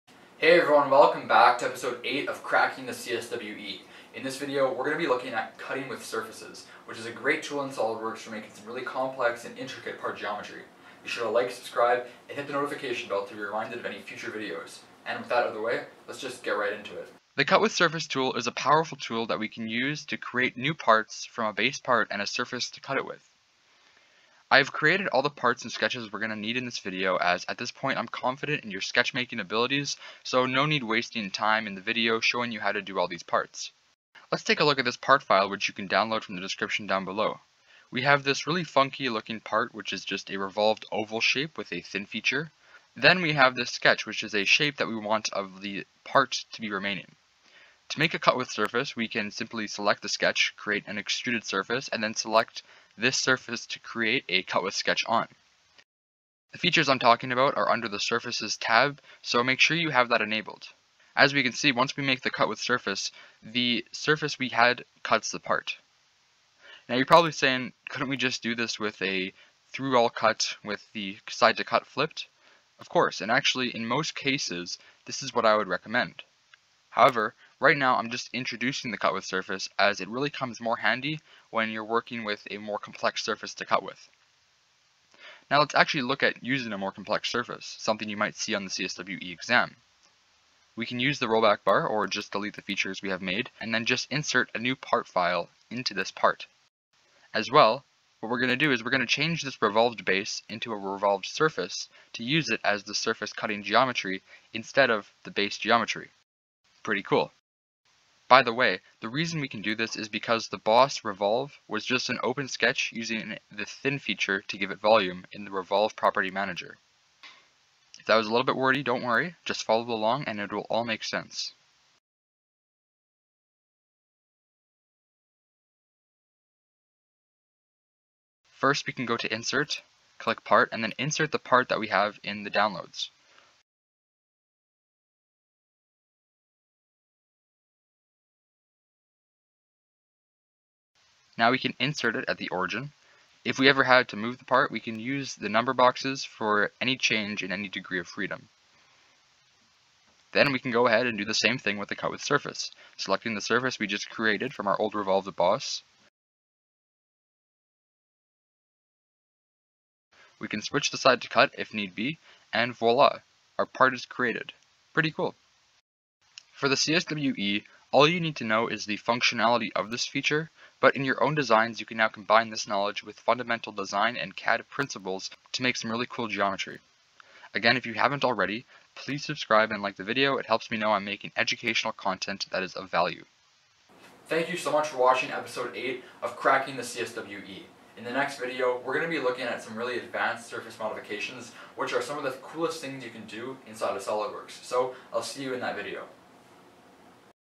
Cracking The CSWE | Episode Eight: Cut With Surface | Full Certified Solidworks Expert Course
Episode eight of Cracking The CSWE, a full series on the CSWE exam. In this video we look at using the cut with surface feature